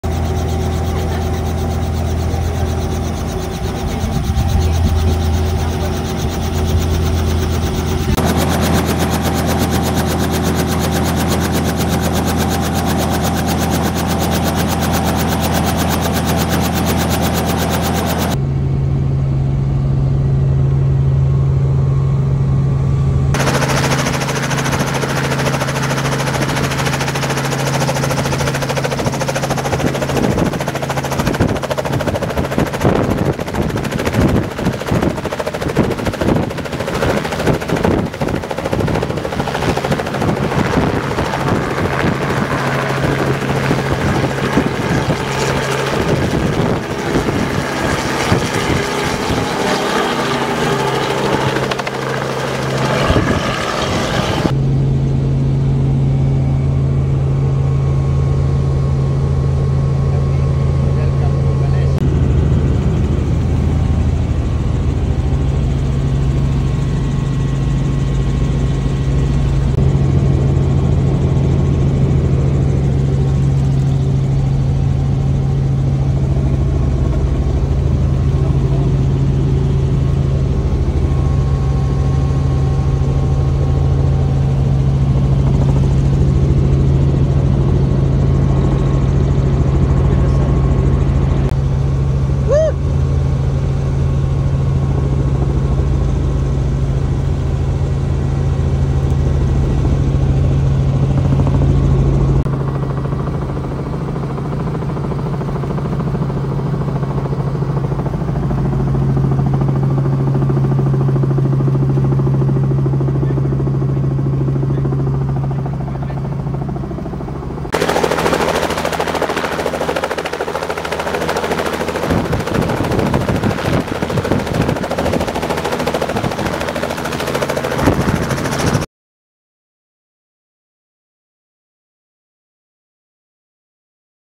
Bird's-Eye View: Discovering Malaysia by Helicopter 🌺🚁
Embark on an enchanting helicopter journey in Langkawi, Malaysia, and behold the captivating beauty of the islands. Soar above the emerald-green landscapes, catching glimpses of pristine coastlines, idyllic islands, and the mesmerizing beauty of a radiant sunset. Experience the allure of Malaysia from the sky, where every twist and turn reveals the stunning contrast of lush greenery meeting the serene blue waters. Witness the splendor of the islands and the ethereal charm of a sunset, making this helicopter ride an unforgettable experience in the heart of Malaysia.